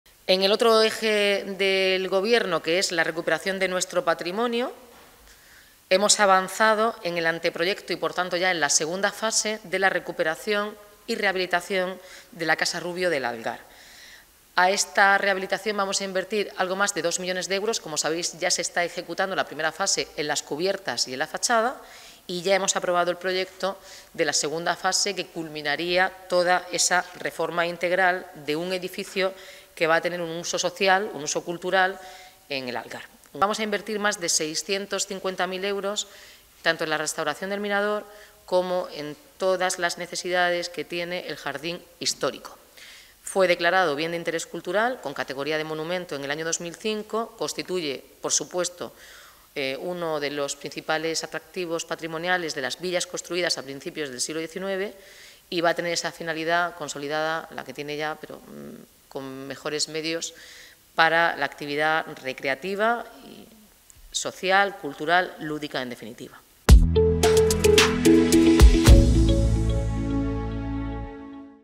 El Ayuntamiento de Cartagena invertirá tres millones de euros para restaurar la Casa Rubio de El ...
El Ayuntamiento de Cartagena invertirá dos millones de euros procedentes de fondos europeos para restaurar la Casa Rubio de El Algar. La alcaldesa Noelia Arroyo ha presidido este jueves, 23 de noviembre, la Junta de Gobierno en la que se ha aprobado el anteproyecto de las obras para la rehabilitación integral de este edificio residencial de estilo ecléctico de finales del siglo XIX en pleno corazón de El Algar.. "Se trata de la mayor inversión que desarrollaremos en un edificio público el año que viene, gracias a fondos europeos, de la Comunidad Autónoma y del Consistorio", ha destacado la regidora.

"La recuperación de la Casa Rubio es una demanda de hace muchos años de los vecinos de El Algar, con los que hemos trabajado para decidir su uso. En la primera planta acogerá los principales organismos de la diputación, como oficinas para la Junta Vecinal, Asociación de Vecinos y la OMITA. Mientras que en la segunda planta se abrirán grandes espacios para la creación de un salón de plenos y un aula de estudio. También se ubicará el Museo Etnográfico", ha resumido la alcaldesa. El montante total del proyecto asciende a 2,5 millones de euros. Ya se ha comenzado a actuar en la zona que más deterioro presentaba este inmueble, las cubiertas.

INVERSIÓN DE 650.000 EUROS EN EL HUERTO DE LAS BOLAS

La Junta de Gobierno también ha aprobado el compromiso de gasto para incluir en el proyecto de presupuestos de 2024 los créditos necesarios para la restauración del mirador y el vallado perimetral del jardín del Huerto de las Bolas, en Santa Ana.  La casa fue diseñada en 1908 por el afamado arquitecto modernista Víctor Beltrí.

"Invertiremos más de 650.000 euros en la restauración del mirador y vallado perimetral del jardín histórico del Huerto de las Bolas. Fue declarado Bien de Interés Cultural, con categoría de monumento, en el año 2005. constituye una de las muchas villas construidas a principios del siglo XIX con finalidad recreativa y productiva", ha concluido Noelia Arroyo.